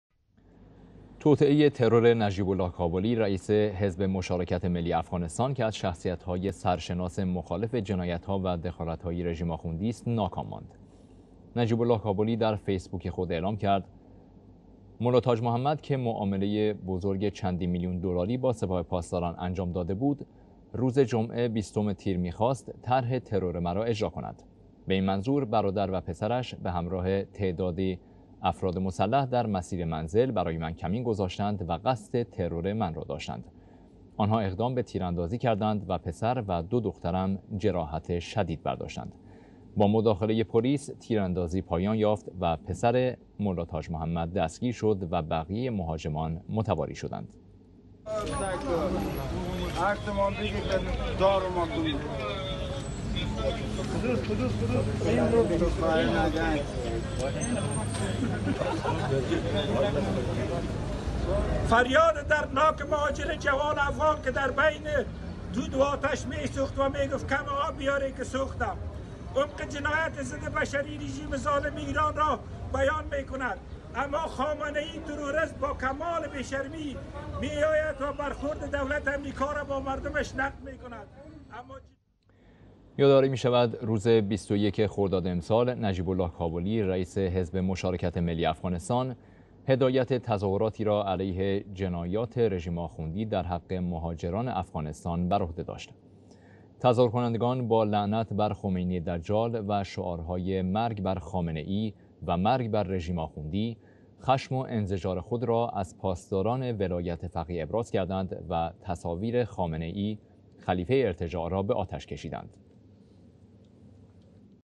شکست توطئه سپاه پاسداران برای ترور نجیب الله کابلی رهبر حزب مشارکت ملی افغانستان
توطئه ترور نجیب الله کابلی رهبر حزب مشارکت ملی افغانستان که از شخصیتهای سرشناس مخالف جنایتها و دخالتهای رژیم آخوندیست، ناکام ماند.

نجیب الله کابلی در  فیس بوک خود اعلام کرد:‌ ملاتاج محمد که معامله بزرگ چند میلیون دلاری   با سپاه پاسداران انجام داده بود،  روز جمعه ۲۰تیر می خواست طرح ترور مرا اجرا کند. به این منظور برادر  و پسرش به همراه تعدادی افراد مسلح در مسیر منزل برای من کمین گذاشتند و قصد ترور من را داشتند. آنها اقدام به تیراندازی کردند  و پسر و دو دخترم جراحت شدید برداشتند. با مداخله پلیس تیراندازی پایان یافت و پسر ملاتاج محمد دستگیر شد و بقیه مهاجمان متواری شدند.

یاداوری می شود روز ۲۱خرداد امسال نجیب الله کابلی رئيس حزب مشارکت ملی افغانسان هدایت تظاهراتی را علیه جنایات رژیم آخوندی در حق مهاجران افغانستان برعهده داشت. تظاهر کنندگان با لعنت برخمینی دجال و شعار های  مرگ بر خامنه ای و مرگ بر رژیم آخوندی خشم و انزجار خود را  از پاسداران ولایت فقیه ابراز کردند و تصاویر خامنه ای  خلیفه ارتجاع را به آتش کشیدند.